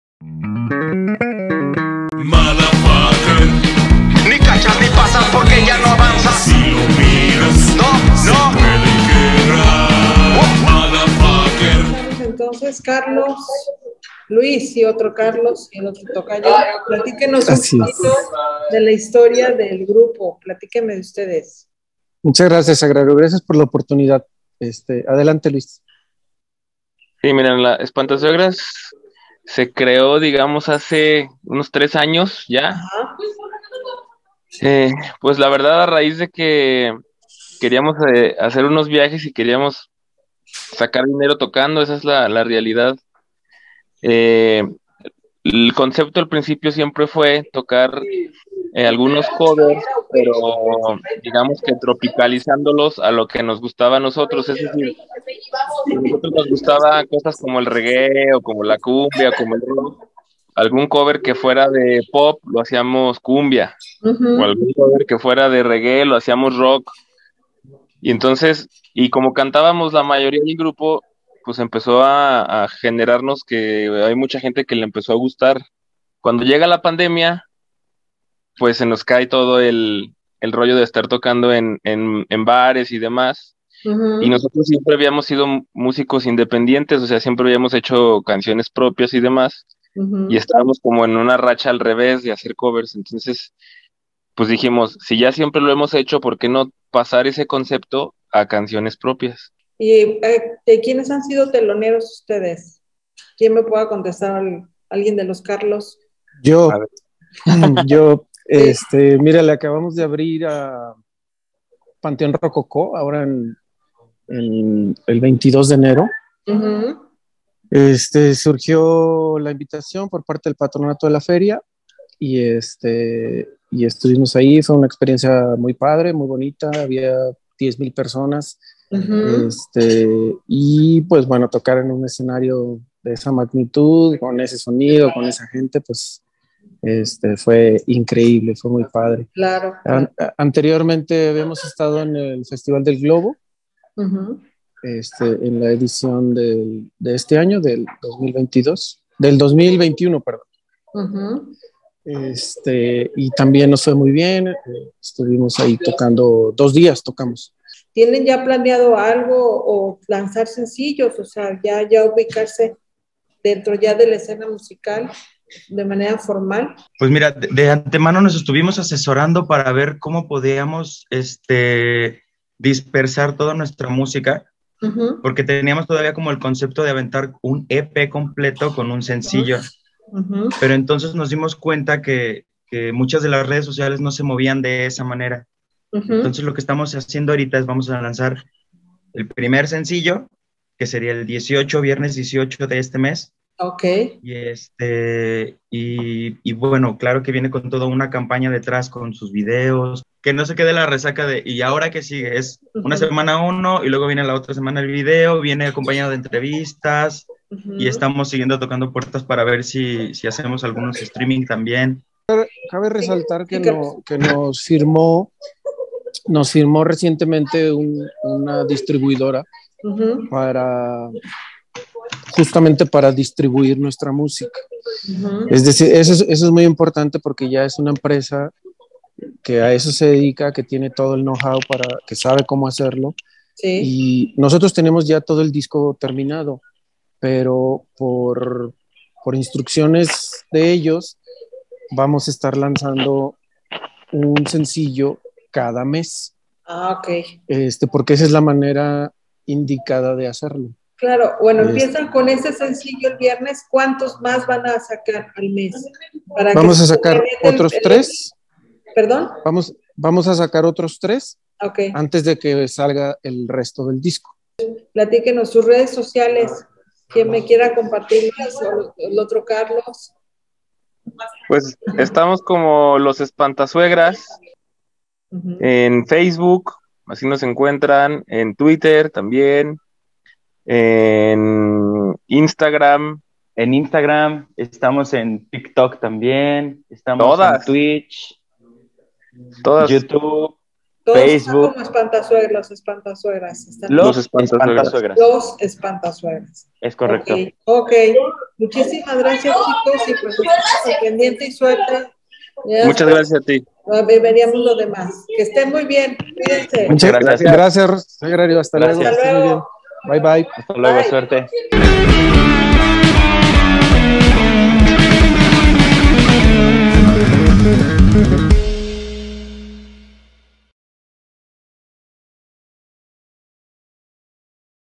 Los Espanta Suegras
Te compartimos un poco de la entrevista que tuvimos con LosEspantaSuegras presentando uno de sus más recientes sencillos.